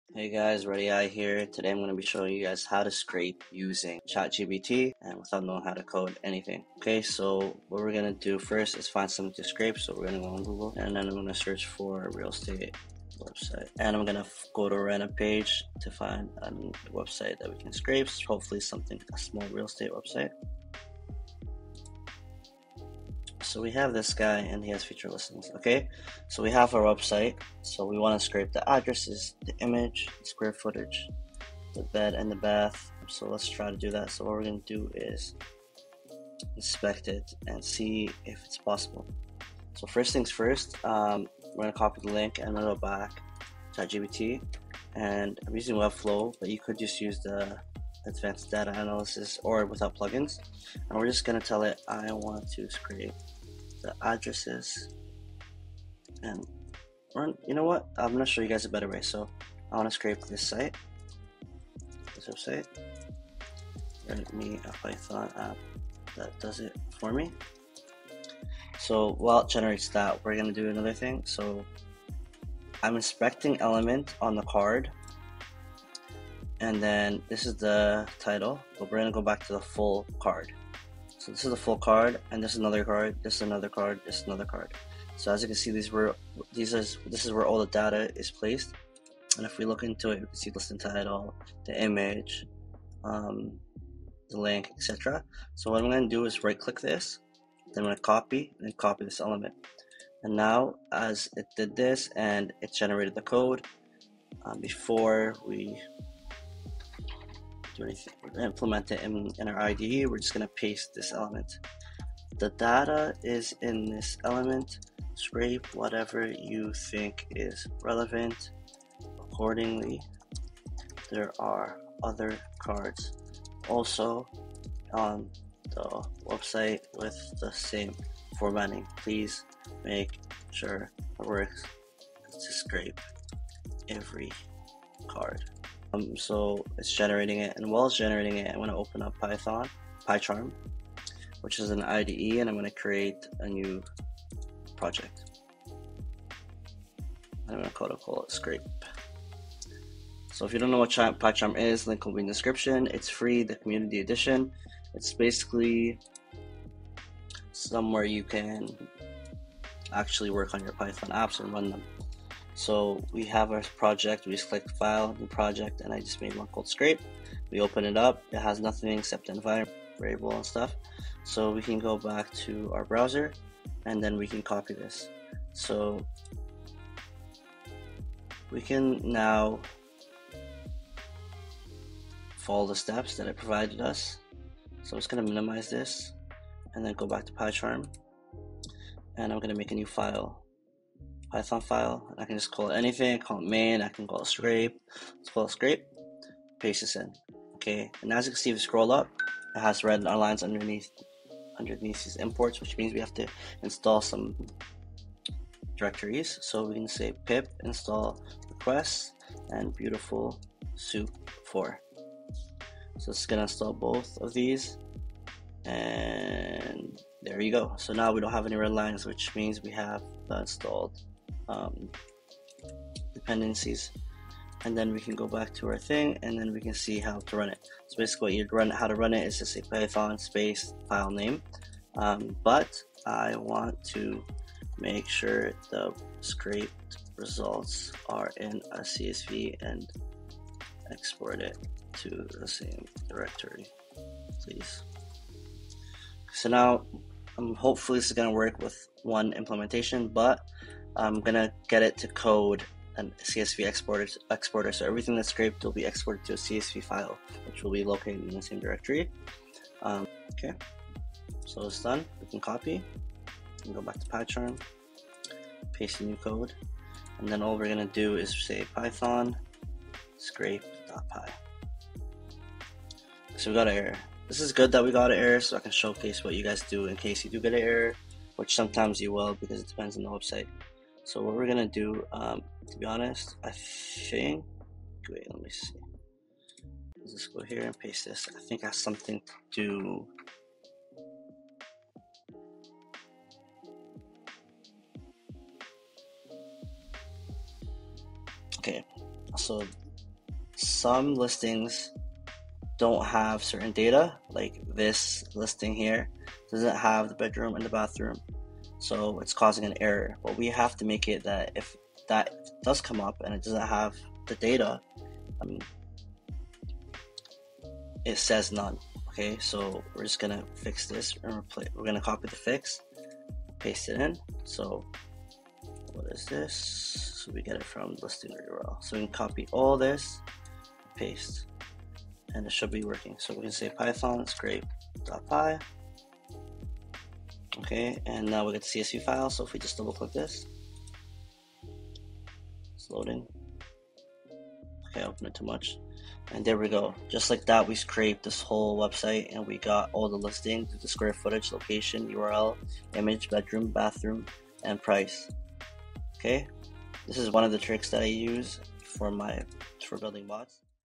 How to Scrape any Website for Free using ChatGPT
In this video I show you how to scrape real estate listings using only chatgpt and python (Just copy and paste the code it generates)